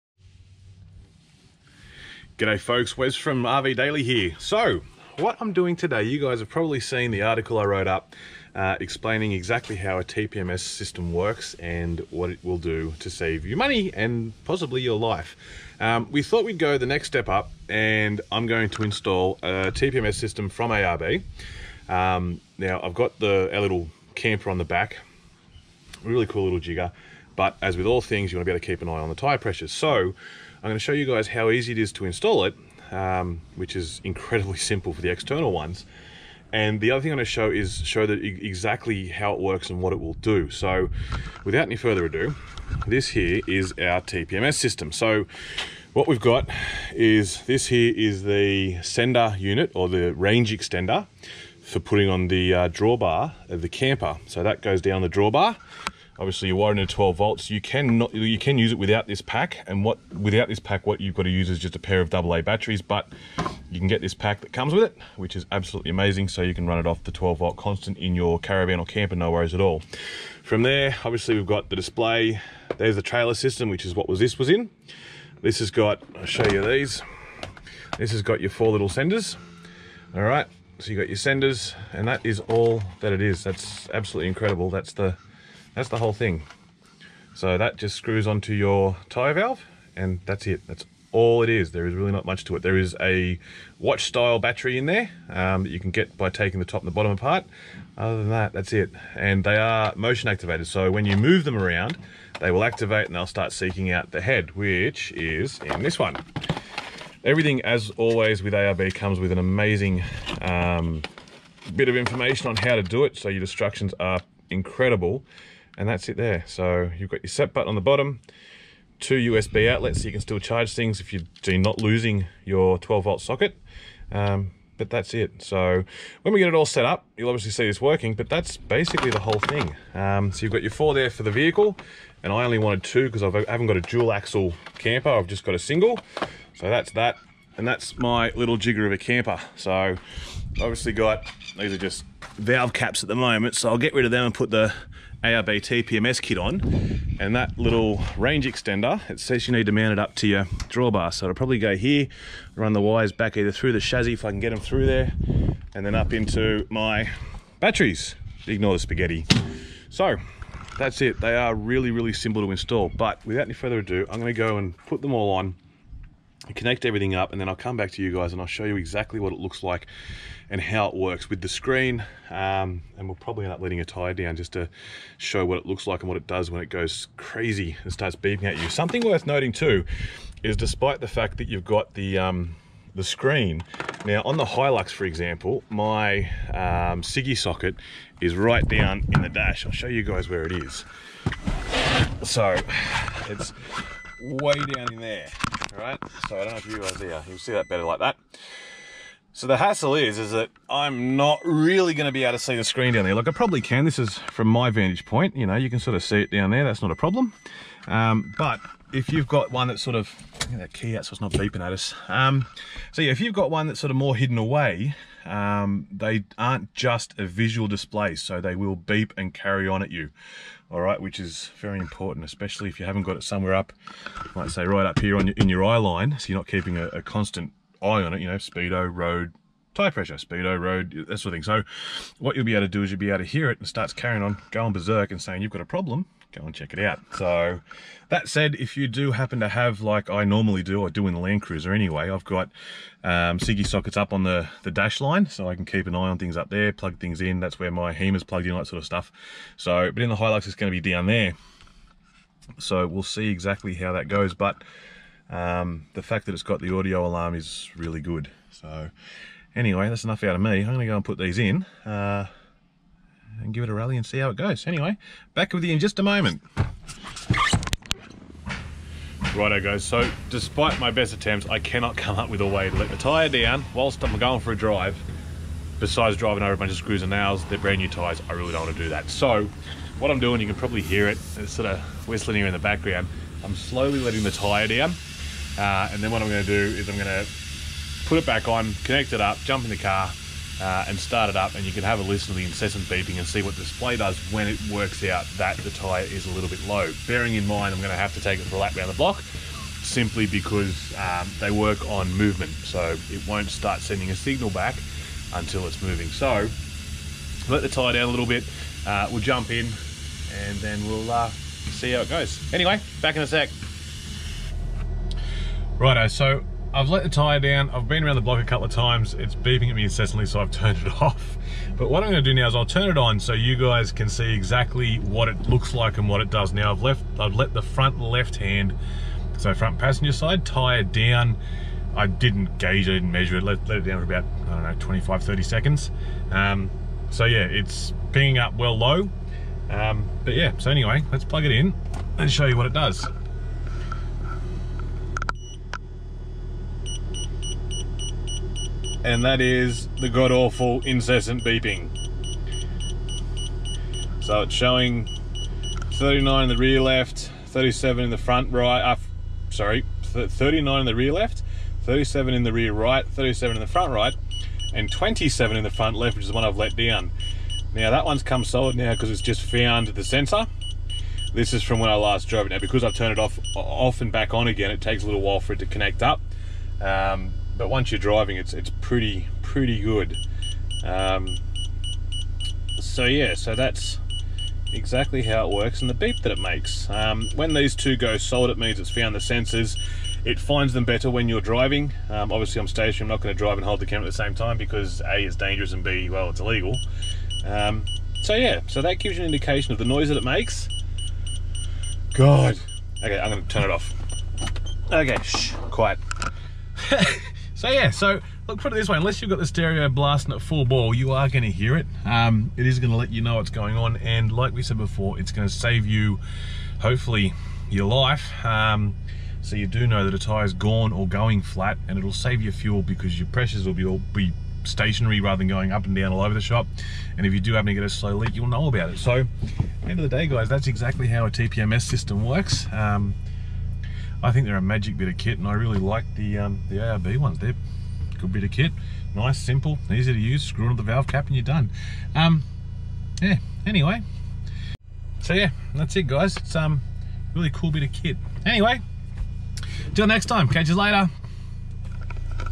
How to install an ARB Tyre Pressure Monitoring System (TPMS)
We give the ARB TPMS a review and shows how these systems work in practice. The video goes with the TPMS story in RV Daily magazine. It goes into more detail about what exactly a TPMS is, and how it can save your life and your back pocket.
In the video, I'll run through the ARB TPMS kit, what's included, how it all connects, how incessantly it beeps at you when you've got a tyre with low pressure, and maybe give you a sneak peek at the camper-trailer I get around in.